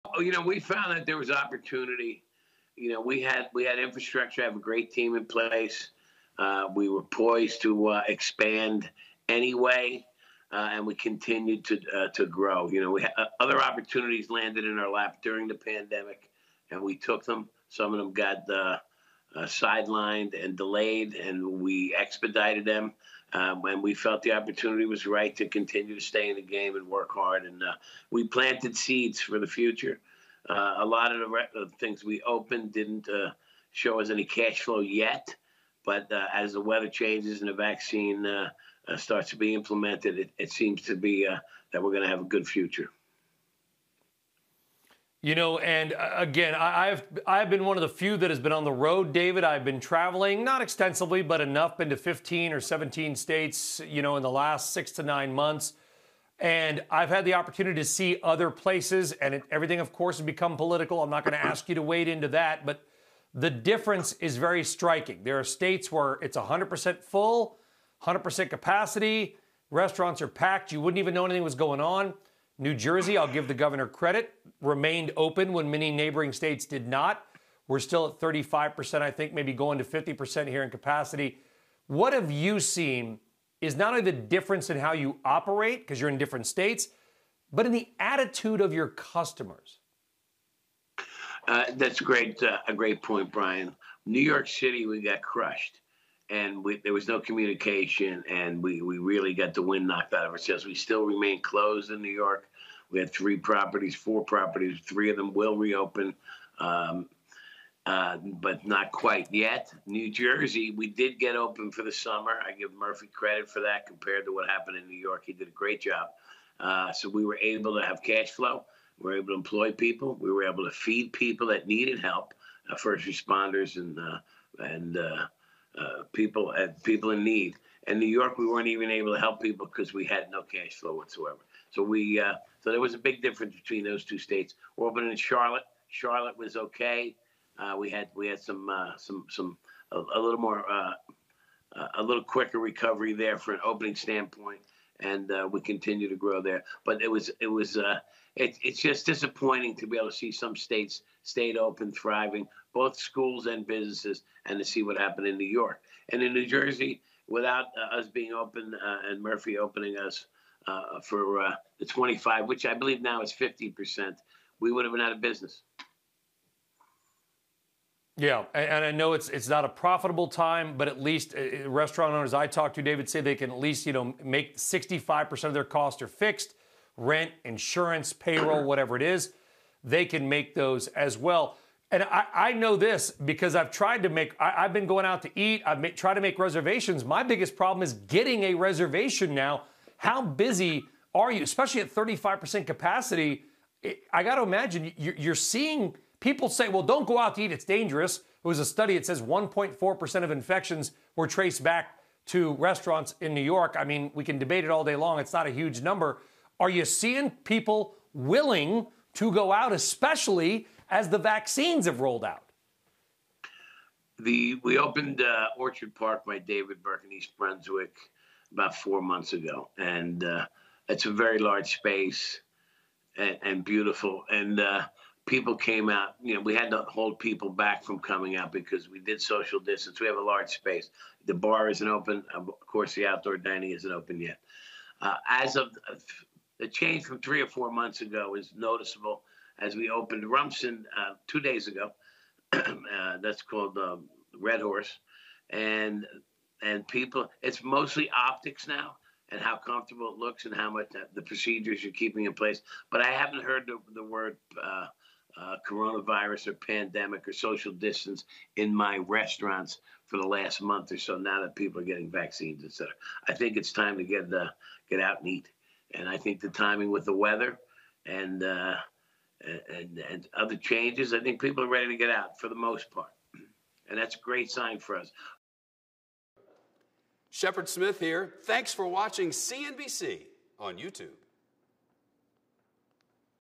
Why highly-respected chef David Burke decided to open restaurants during the pandemic
David Burke, chef and owner of 18 restaurants in New Jersey and several other parts of the country, thinks the pandemic will ultimately help restaurants located in the suburbs.